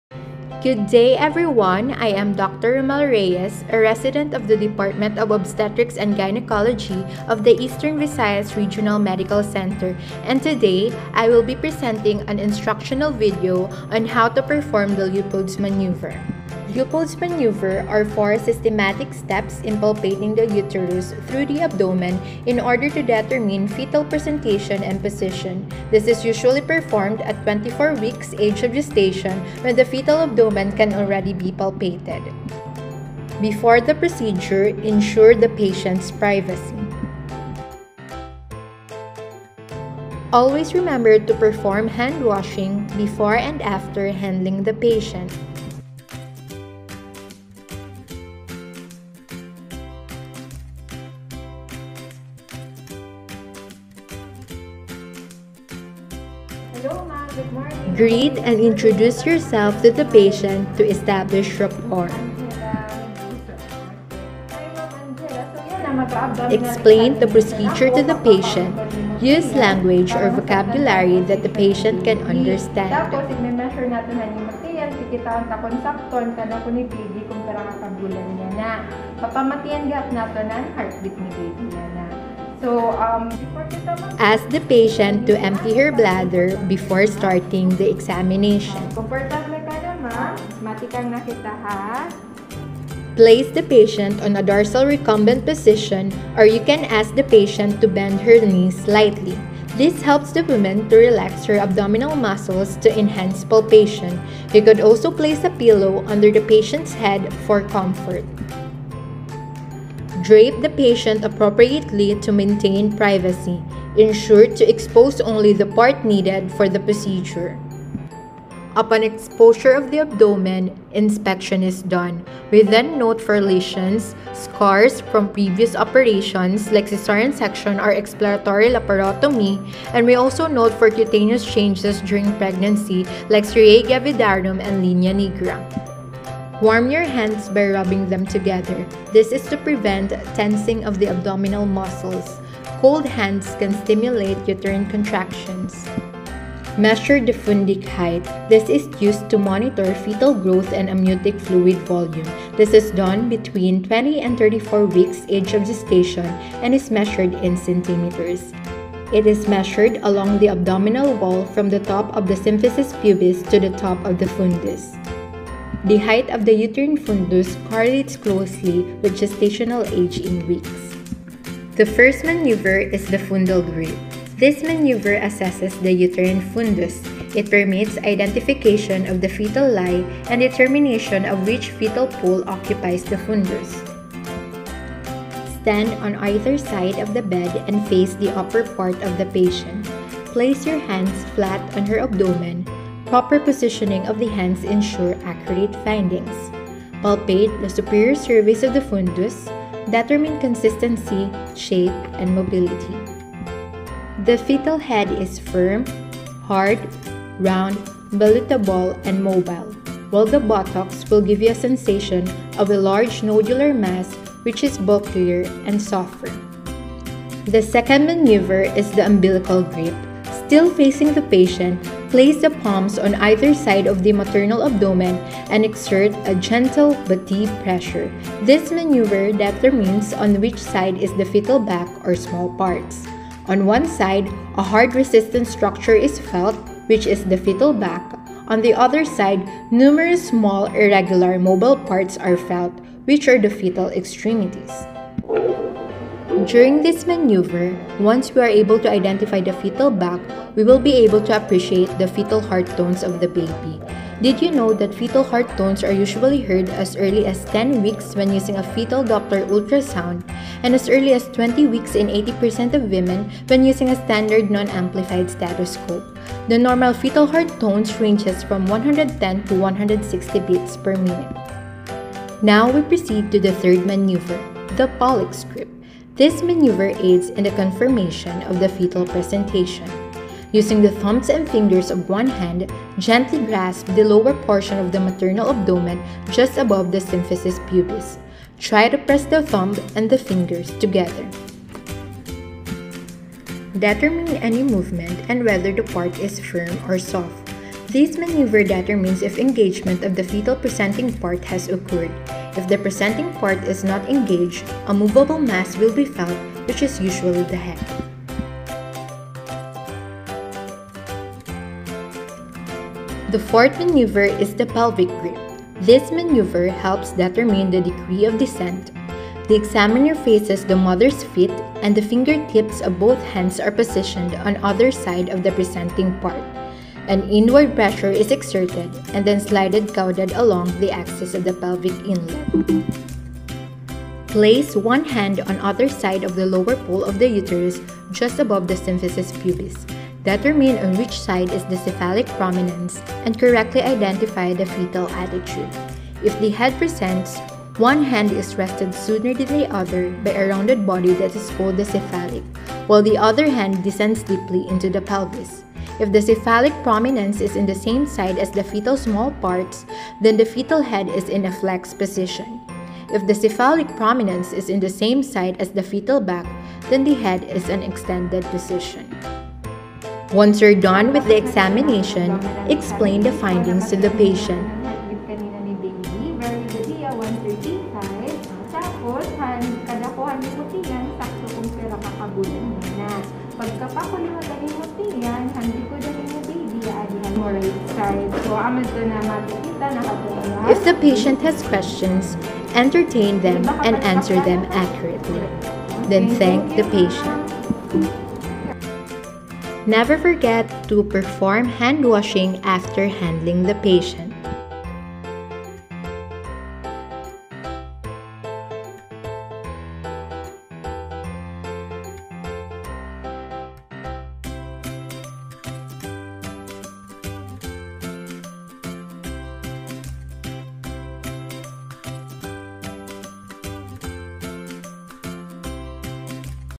LEOPOLD'S MANEUVER
This is an instructional video on how to perform an abdominal examination in a pregnant patient which  includes the Leopold's Maneuver.